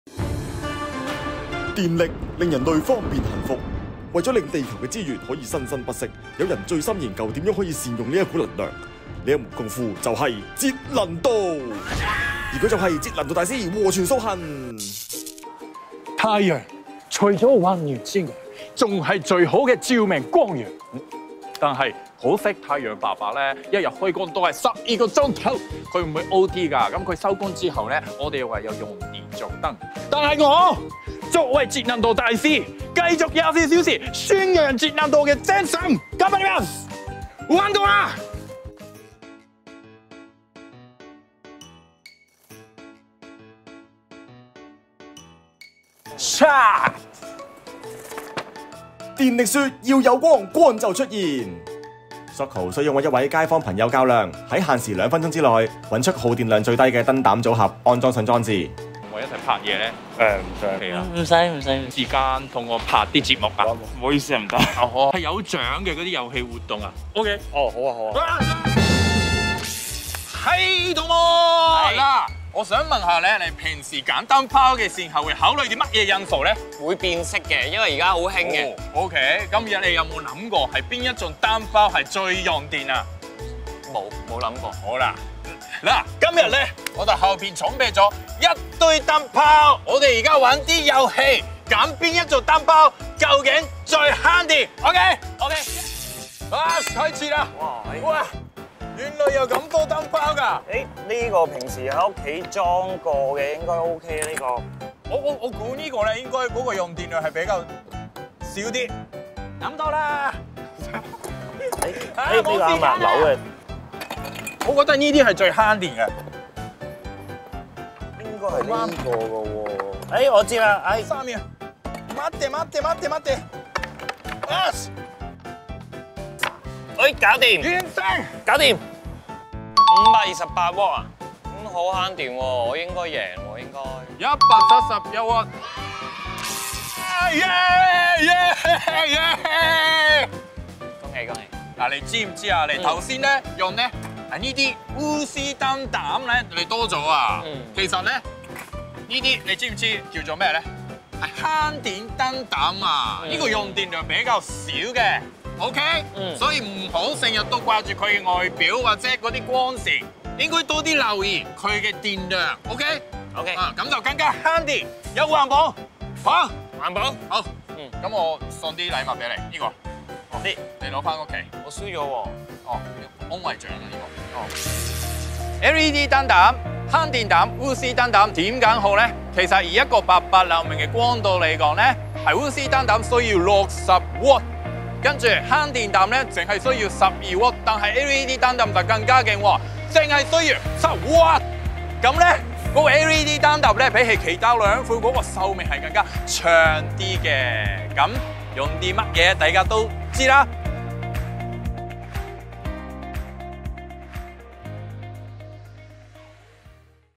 【💡追尋節能之道📜: 深水埗】
太陽伯伯為我哋帶來溫暖同照明，但係佢係會休息同埋唔會加班㗎，所以我哋就需要電燈幫幫手！

燈膽有咁多種，你又知唔知邊種最慳電先😱？

Soko San今次喺深水埗美化配電箱搵到第三式，同街坊一齊於時限內搵出最慳電嘅燈膽！過程係點？快啲去睇片啦！如欲了解更多有關中電配電箱美化計劃的資訊，請瀏覽